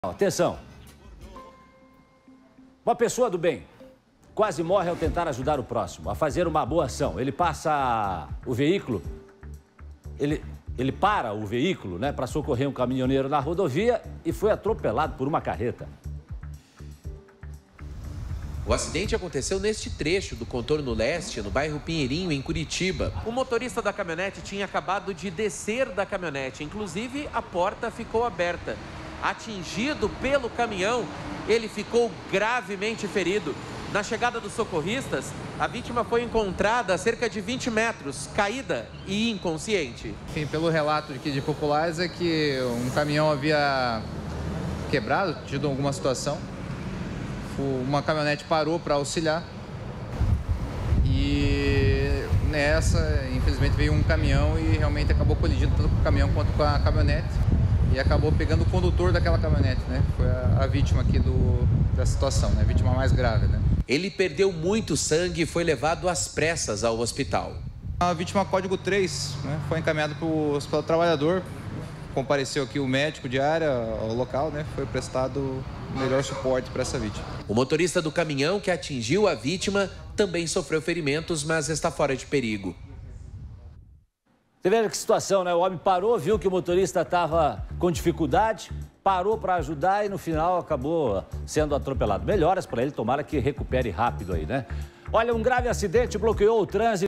Motorista desce para ajudar caminhoneiro e é atropelado por uma carreta
Uma pessoa do bem, quase morre, ao tentar ajudar o próximo. Ele para o veículo para socorrer um caminhoneiro na rodovia e foi atropelado por uma carreta. O estado é grave. Confira todos os detalhes na reportagem!